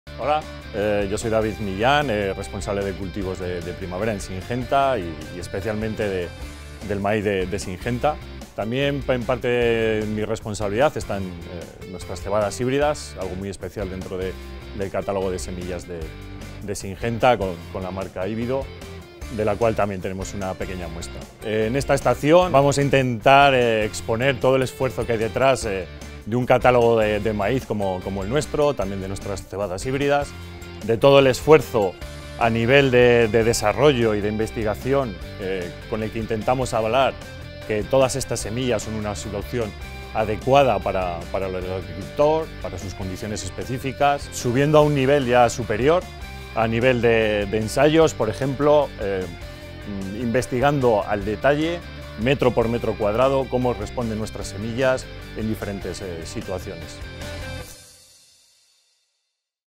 Entrevista a David Millán
Entrevista a David Millán, sobre las novedades de Syngenta para los cultivos de maíz y cebadas híbridas.